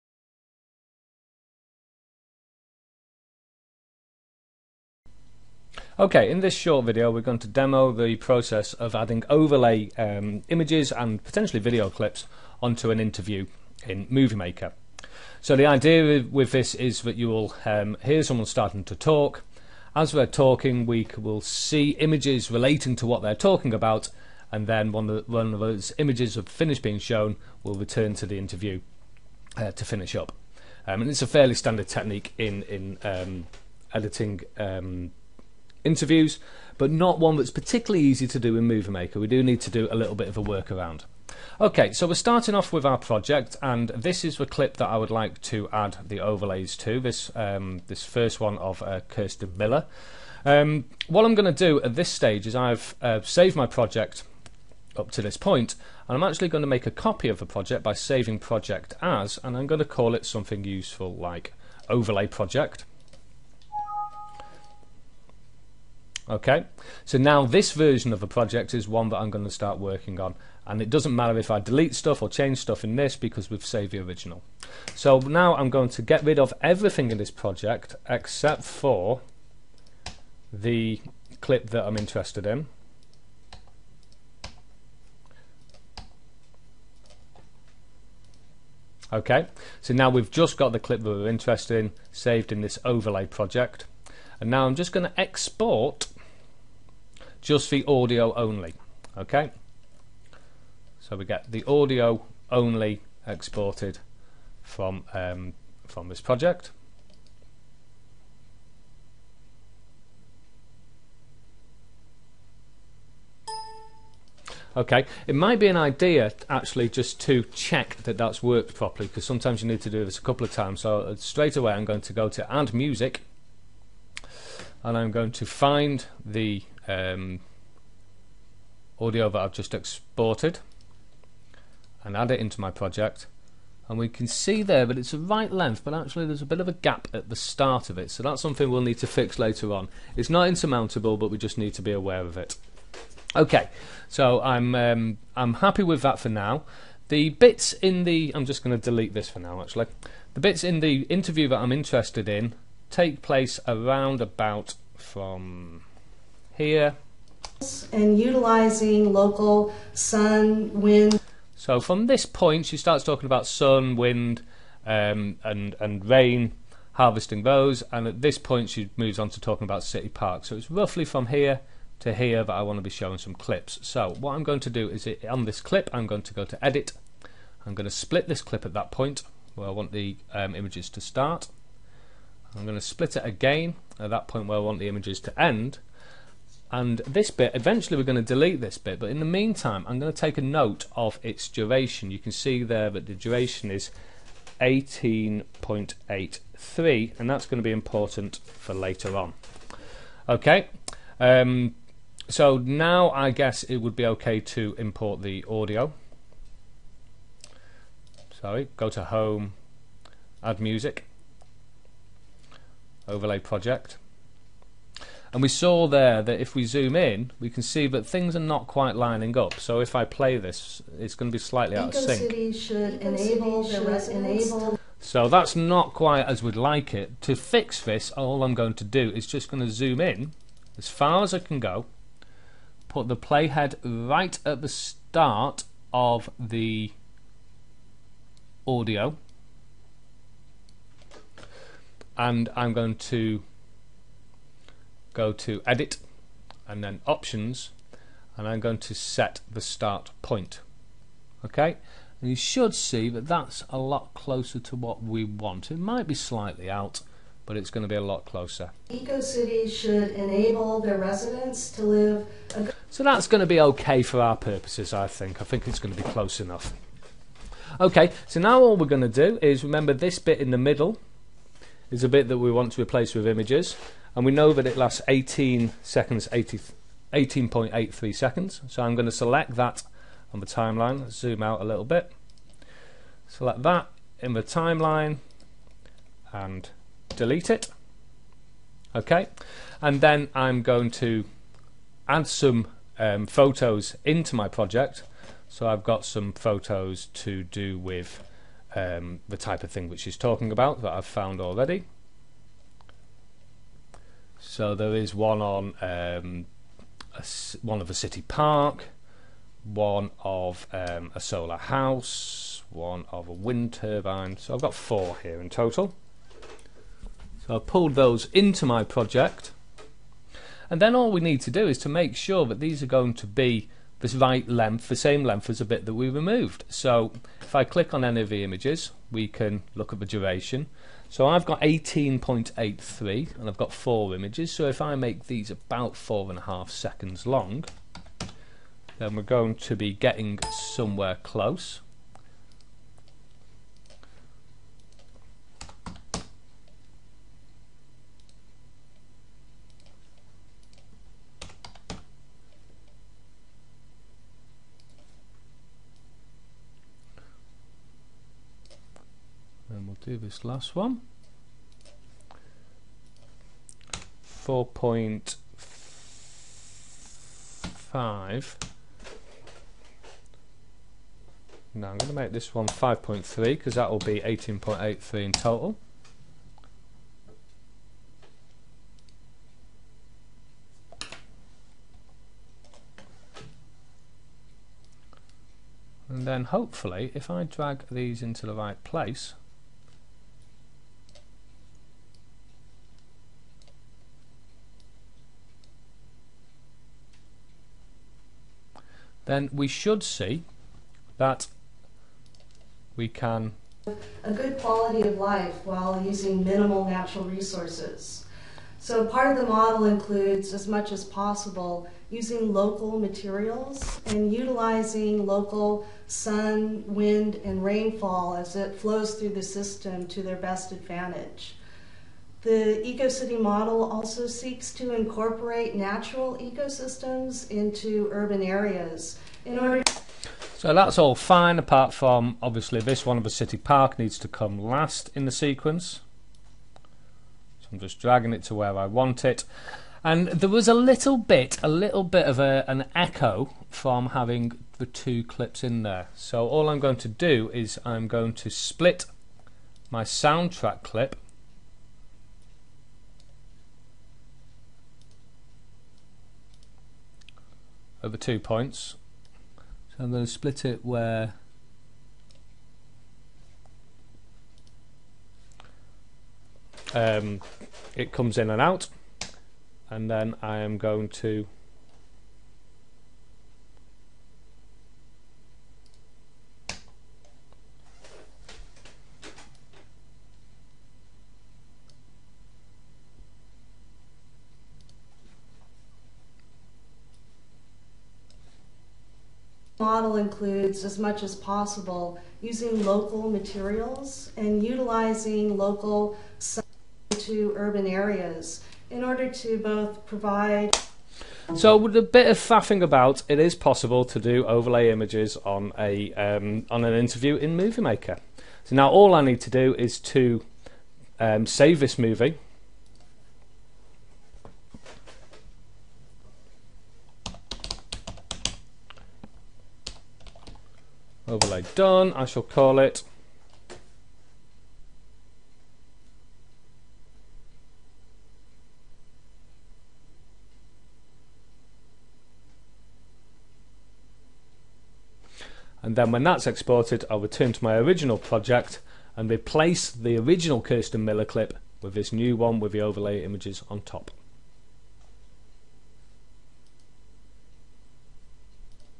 Adding overlay video in MovieMaker
Rough tutorial demonstrating the process for adding overlay images to video footage in MovieMaker.